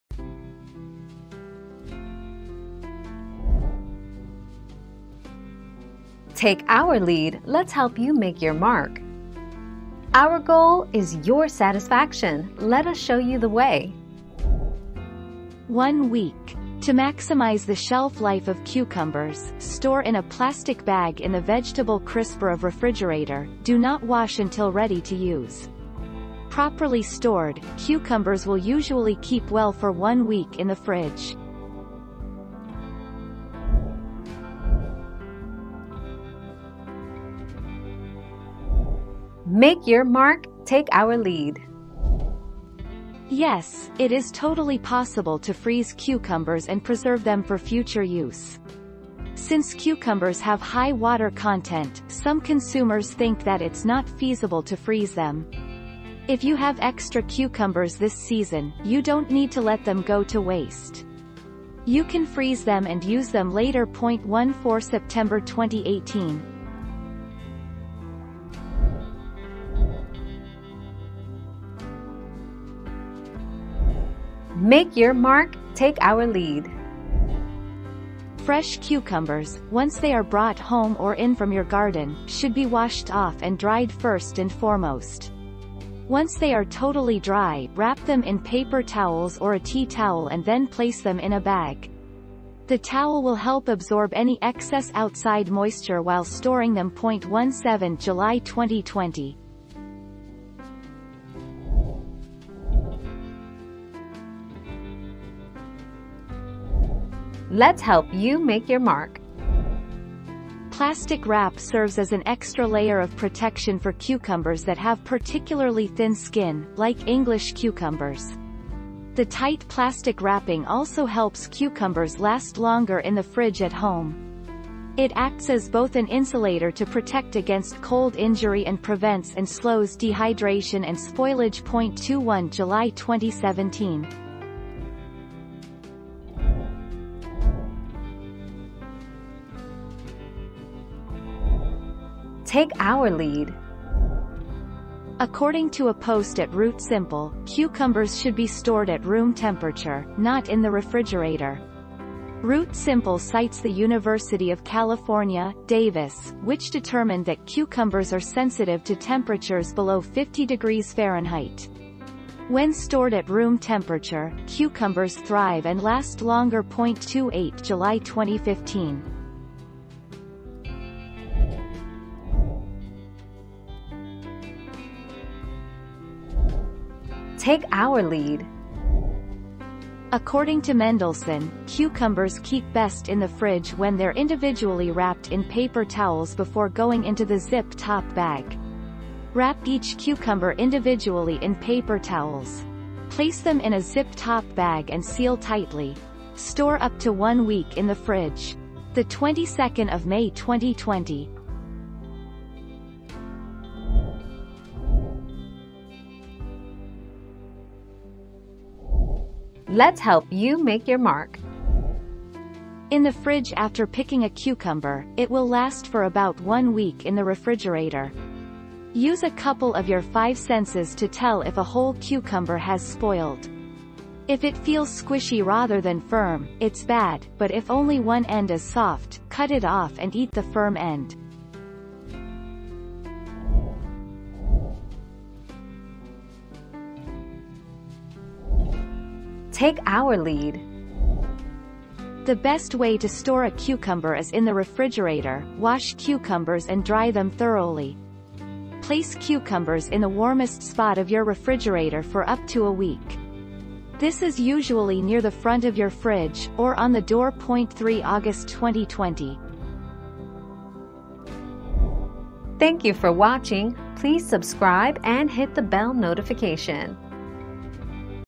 How long can I keep cucumber in the fridge?
00:00 - How long can I keep cucumber in the fridge?
00:35 - Can you freeze cucumbers for later use?
01:10 - How do you keep cucumbers fresh without a refrigerator?
01:43 - Why are English cucumbers wrapped in plastic?
02:17 - Should cucumbers be kept in the fridge?
02:52 - How do you store cucumbers long term?
03:24 - Do cucumbers go bad in the fridge?
03:54 - What is the best way to store cucumbers?

Laura S. Harris (2021, March 11.) How long can I keep cucumber in the fridge?
    AskAbout.video/articles/How-long-can-I-keep-cucumber-in-the-fridge-227706